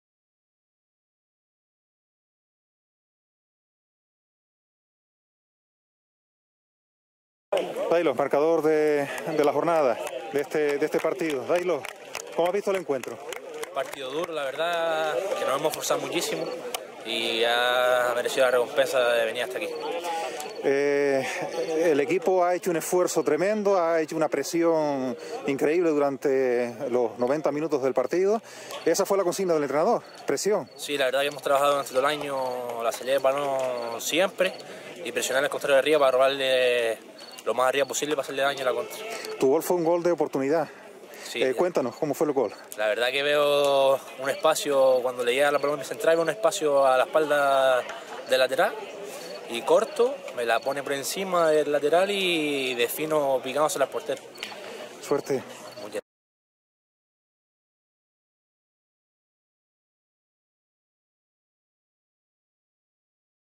Comentario de Dailos, jugador de la UD San Antonio. Partido UD Teror-UD San Antonio 29 4 2016
Comentario post partido de Dailos, jugador de la UD San Antonio,
tras el encuentro UD Teror-UD San Antonio. Regional Preferente Las Palmas. Temporada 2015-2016. Jornada 31. 29/4/2016. Campo de Fútbol Municipal del Pino. Teror. Resultado final: 0-1.